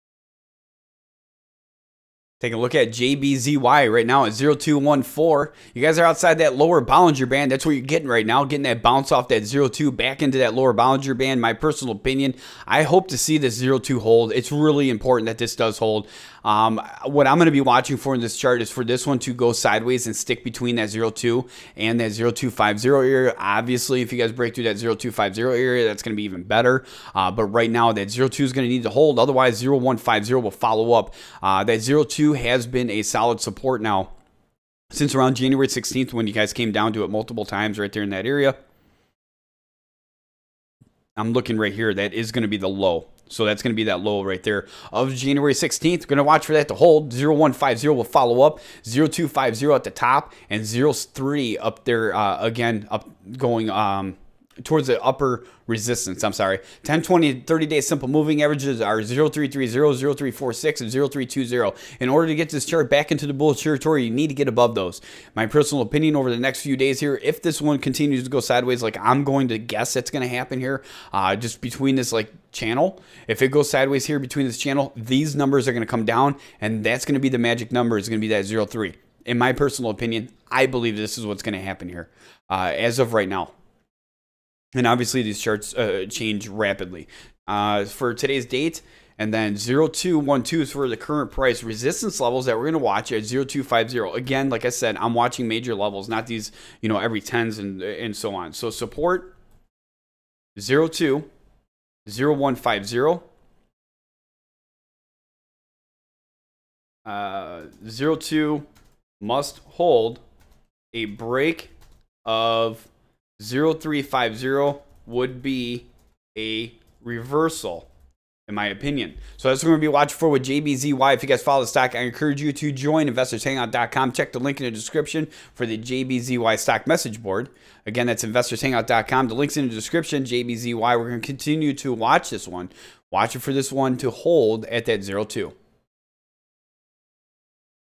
JBZY Stock JB&ZJMY Holding Company Inc. Live Analysis 02-20-2018
JBZY Stock JB&ZJMY Holding Company Inc. Live Analysis as of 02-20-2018. Today we take a look at the current stock price and stock chart for $JBZY.

If you are interested in others opinions, filings, financials, news or any other information about JBZY be sure to check out the Investors Hangout Stock Message Board:

I am the Founder/Owner/Admin of Investors Hangout.